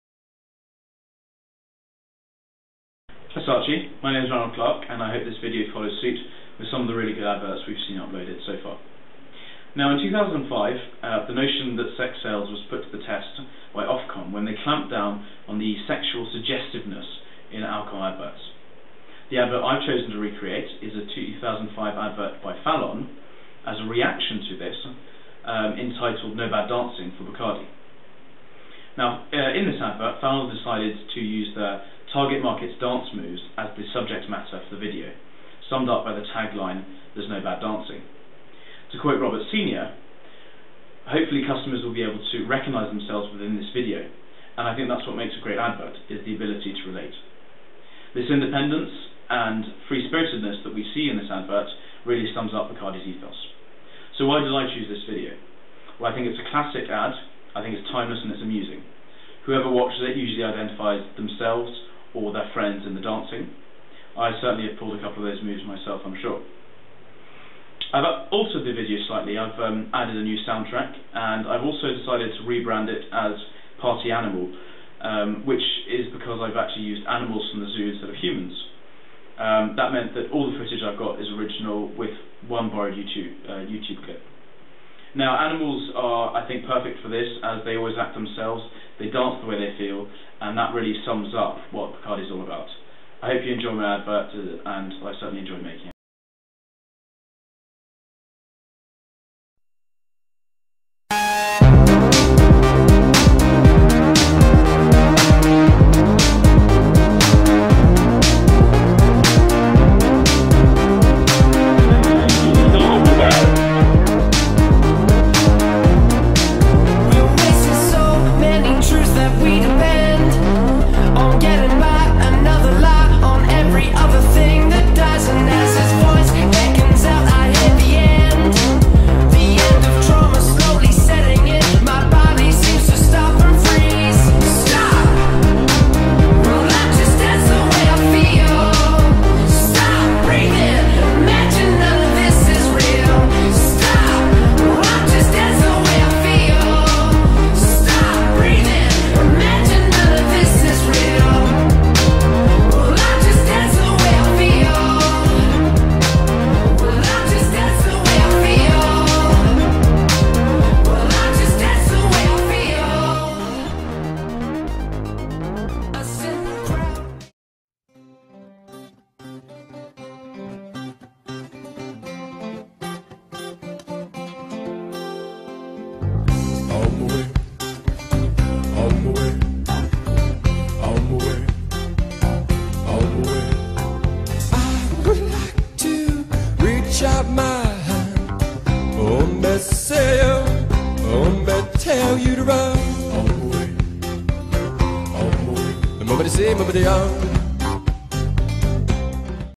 Ranald Clarke - Saatchi Brief 3 - Bacardi Advert
This is my take on the No Bad Dancing ad for Bacardi by Fallon.